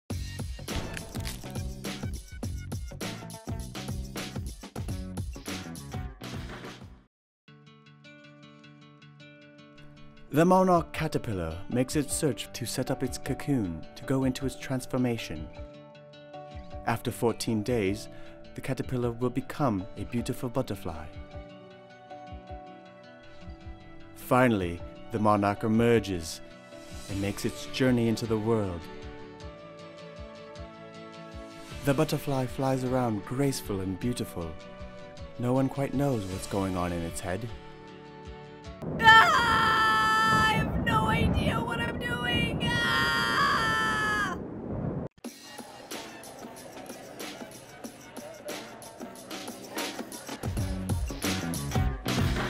THE BUTTERFLY
The fascinating life of the Monarch butterfly

Voices by Sam Venti
                  Erik Steele